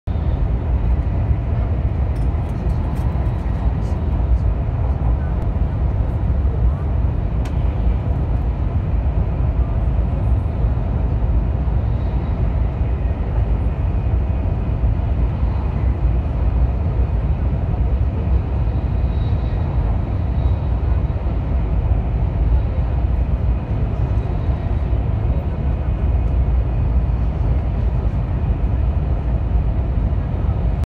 高速造成景物扭曲
偽科學的廣義相對論

速度越快時間越慢

高速移動造成景物扭曲

所有正方形都變成了平行四邊形